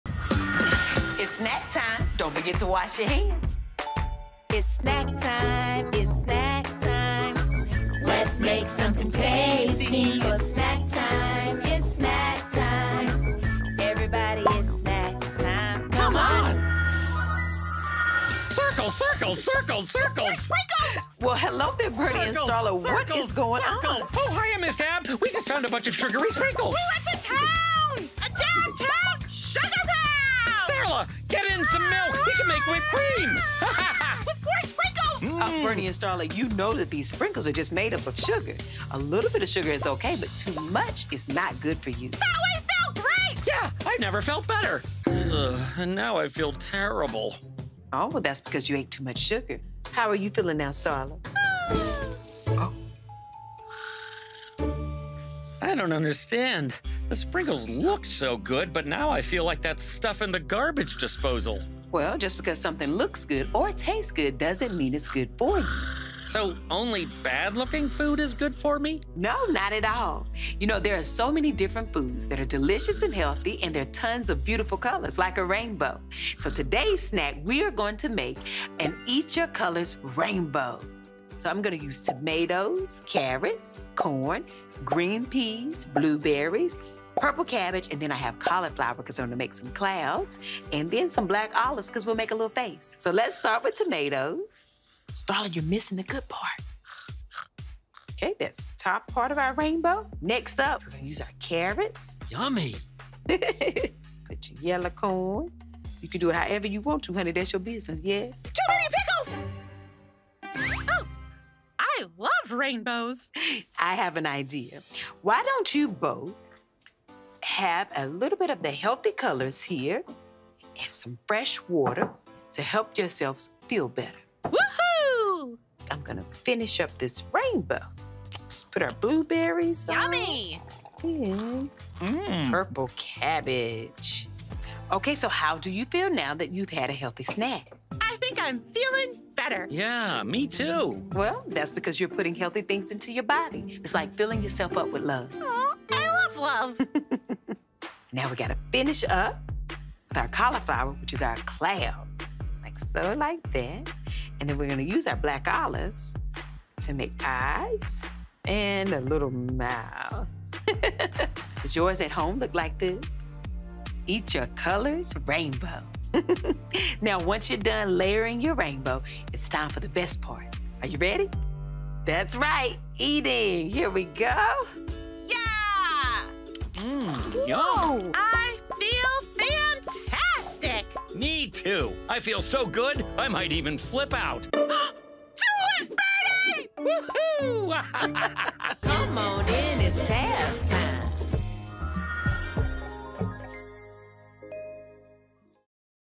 Snack Time: Eat-Your-Colors Rainbow | Healthy Snacks for Kids | Cartoons for Kids | Toddlers
We all need to take care of ourselves even Ms. Tab, Burnie the Oven Mitt, and Starla the Spatula. So when Ms. Tab (Tabitha Brown) finds Burnie (John Michael Higgins) and Starla (Ashley Nicole Black) in the kitchen having eaten too much sugar she suggests they make a super healthy, fun and colorful snack called Eat-Your-Colors Rainbow. Not only is this a delicious snack that will make you feel good, but it also has a ton of beautiful colors!

Ingredients
Tomatoes
Carrots
Corn
Green Peas
Blueberries
Purple Cabbage
Cauliflower
Black Olives